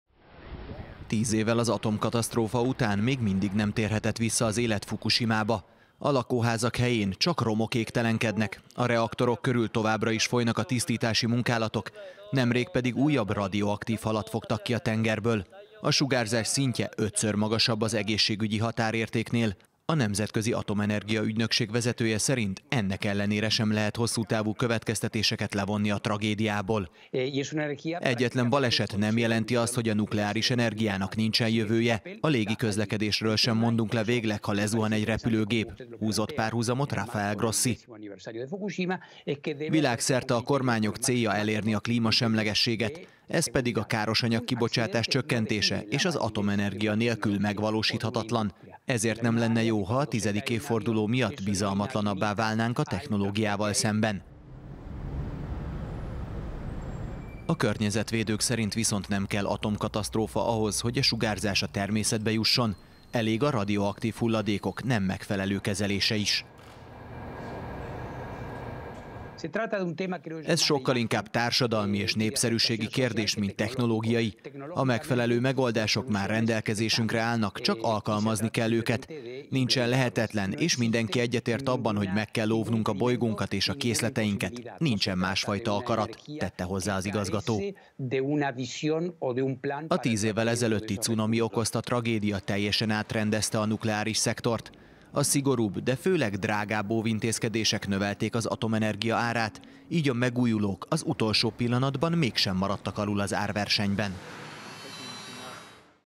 IAEA: a fukusimai katasztrófa után is bíznunk kell az atomenergiában
Az ügynökség igazgatója az Euronewsnak adott exkluzív interjút a japán tragédia 10. évfordulója alkalmából.